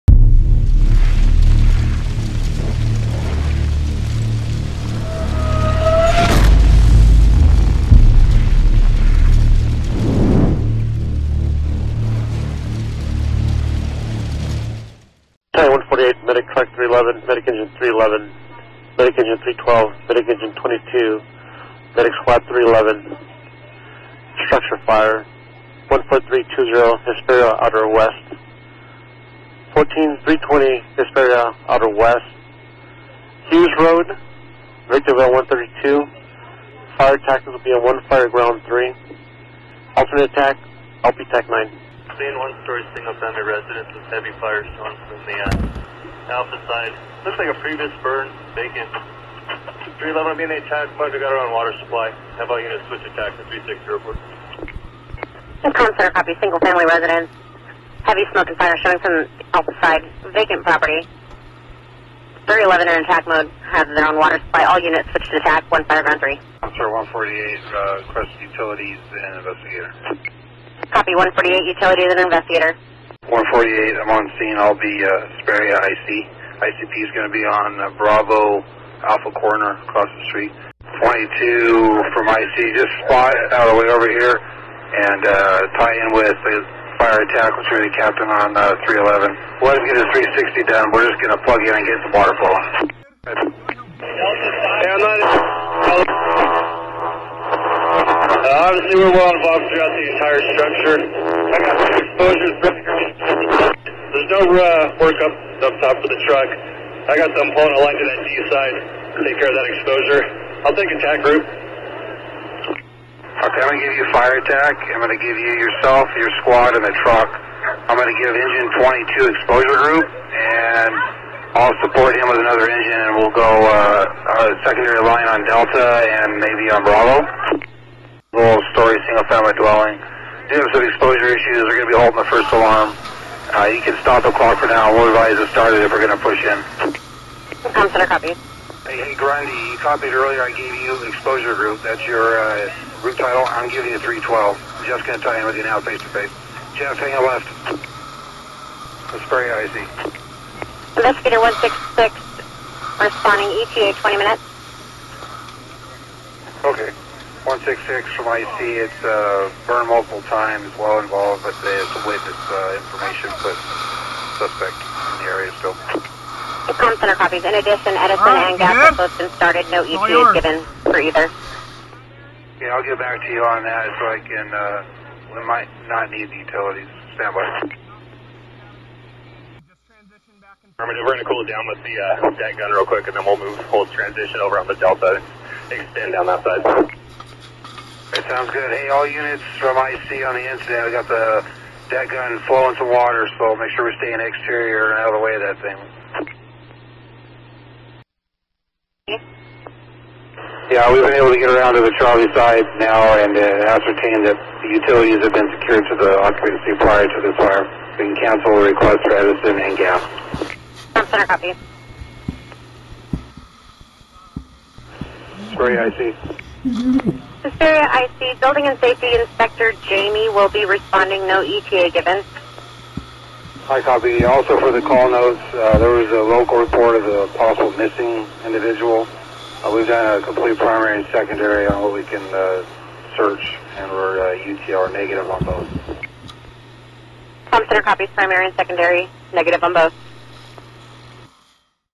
Residential Structure Fire Outer Hesperia Road Division 6 City of Victorville
On August 14th, 2018 at 7:31PM SBCoFD responded to a residential structure fire in the 14k block of outer Hesperia Rd in the City of Victorville. Medic Engine 311 arrived in 4 minutes and reported a well involved single story vacant home. Initial priorities were to protect surrounding structures and contain the fire to the building of origin. Firefighters initiated a defensive attack with a deck gun along with multiple hand lines to protect exposures. The fire was ultimately declared out in approximately 30 minutes. San Bernardino County Fire responded with 3 paramedic engines, 1 truck company, 1 medic squad, 1 battalion chief and 1 investigator for a total of 16 personnel. There were no injuries to civilians or fire personnel. The cause is currently under investigation.